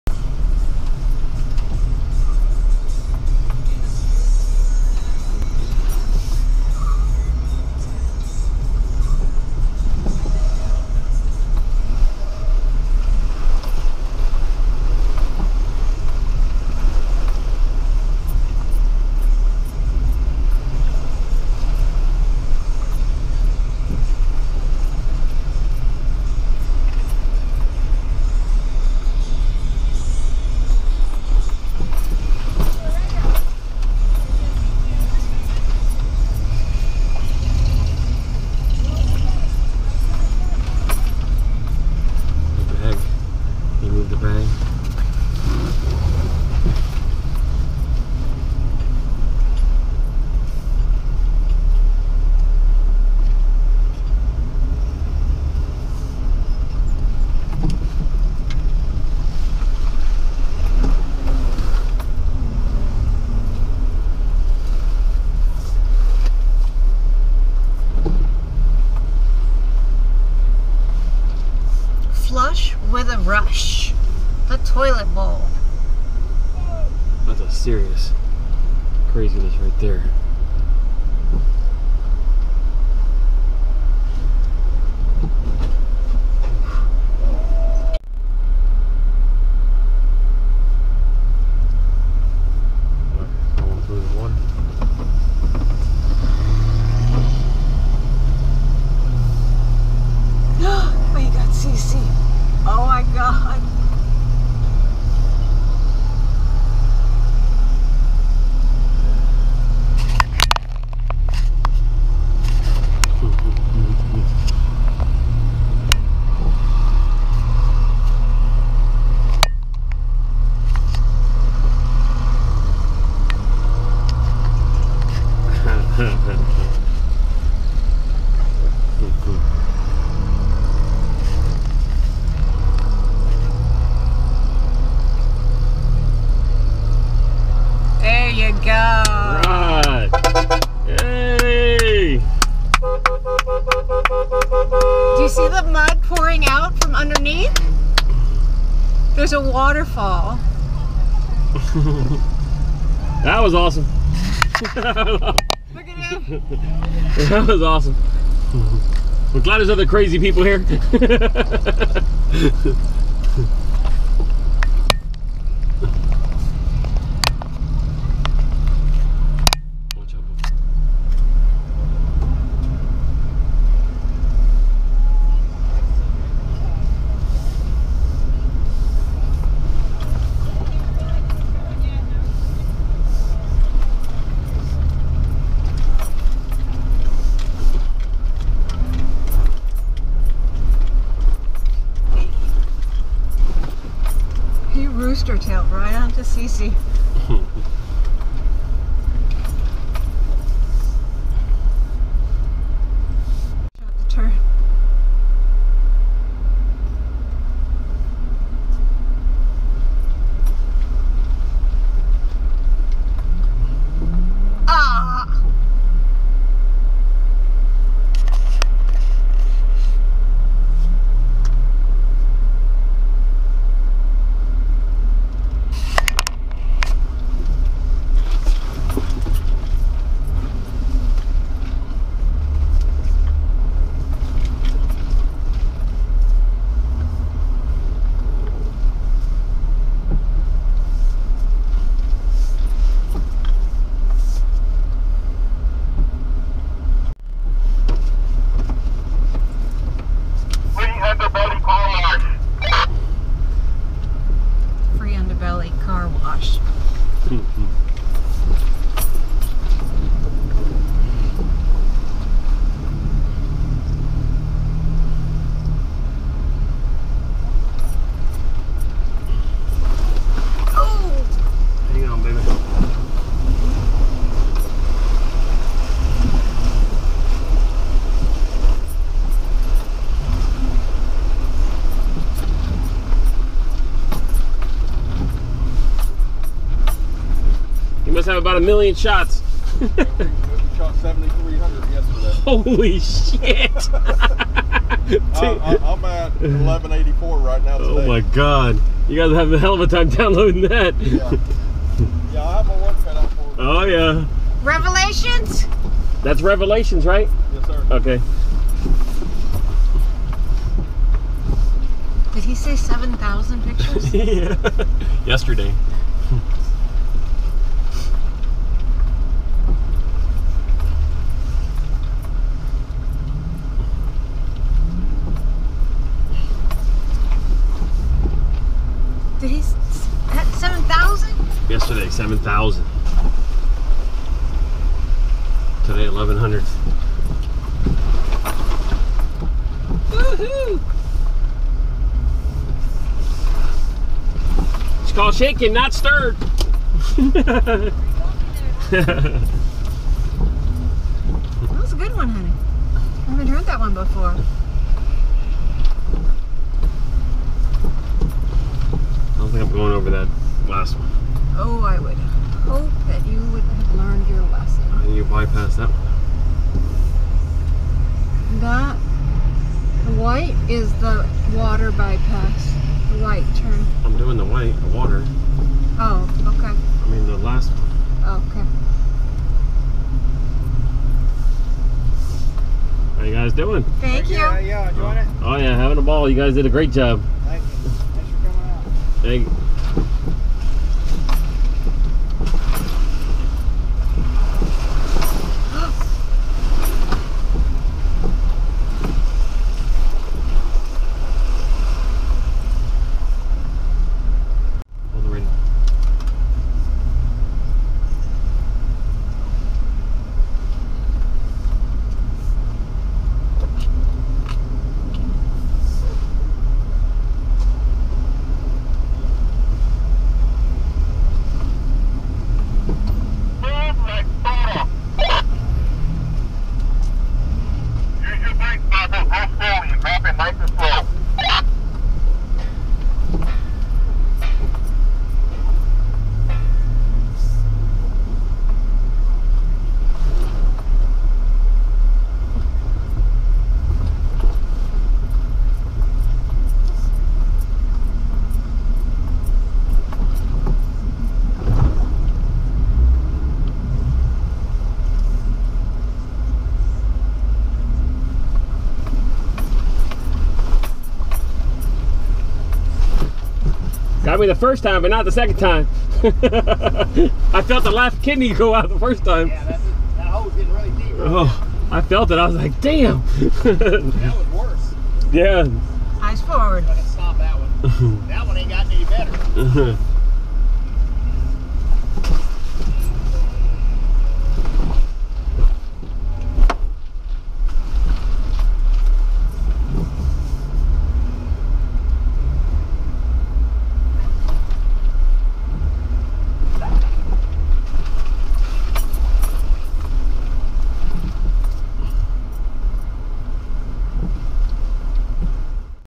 Jeeptoberfest Stock Course 2016
Ocala Jeeptoberfest puts on a thrilling Halloween-themed off road obstacle course. Plunge into the Toilet Bowl for a flush with a rush. Witness the extreme Carnage Canyon. Cheer on the fool on the mud run. Climb Hell's Hill. Beware of Alex's sand spray. Dive into the Witches Brew.